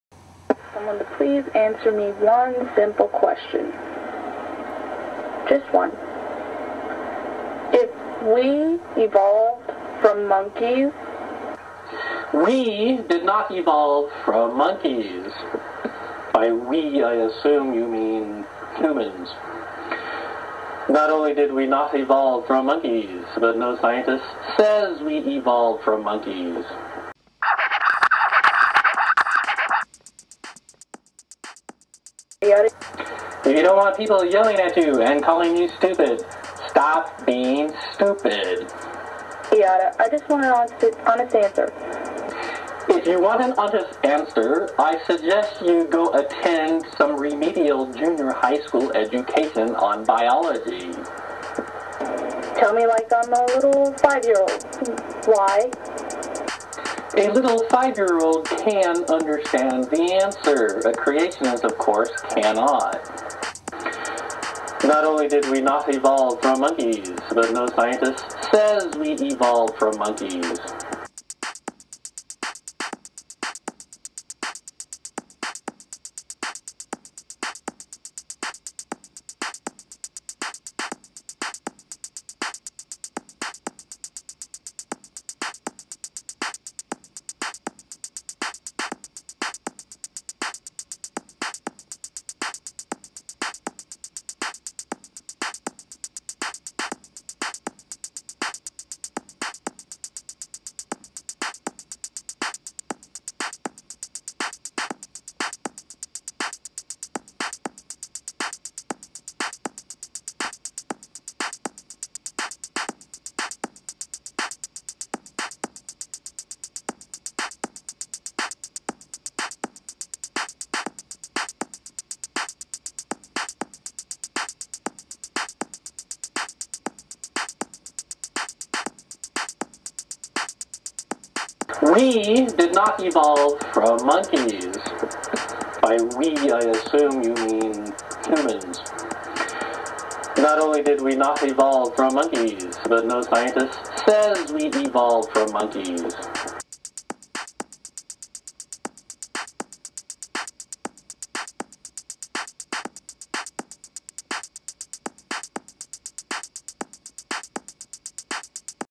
Desertphile: wrong on evolution from monkeys?
ScienceCheck looks into Desertphile's claim, and we found an unusual answer.